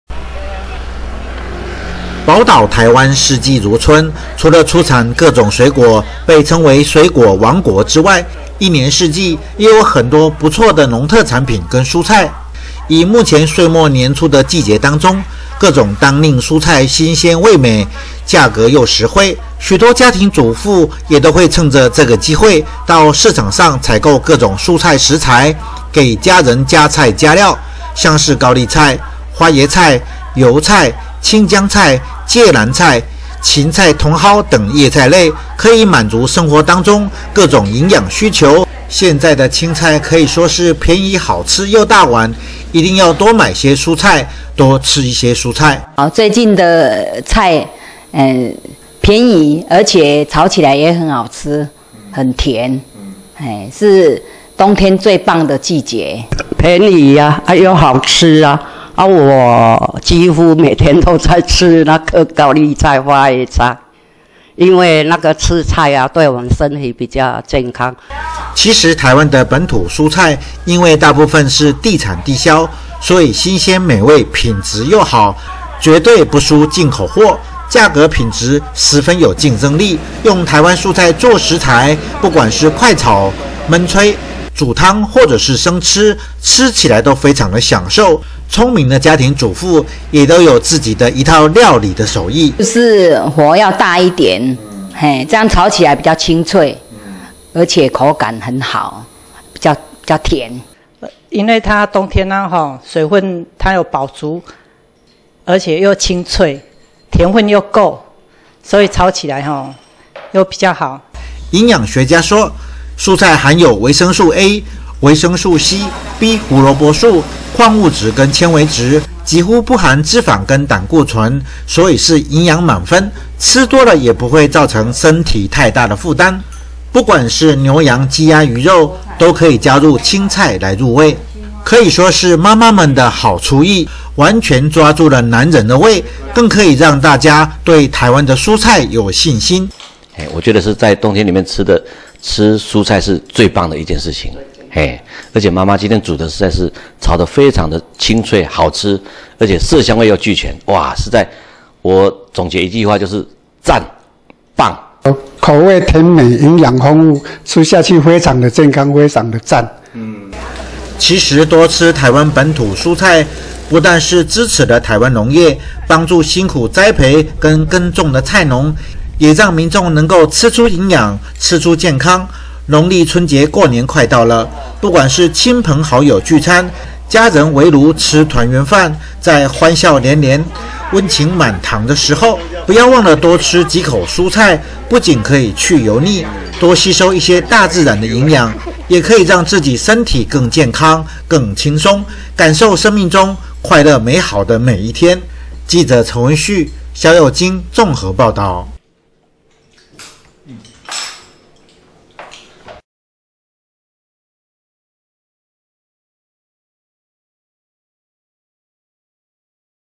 《台傳媒新聞》20190123 當季蔬菜美味新鮮營養 歲末團圓聚餐輕鬆上菜
【記者 陳文旭、蕭又菁 / 綜合報導】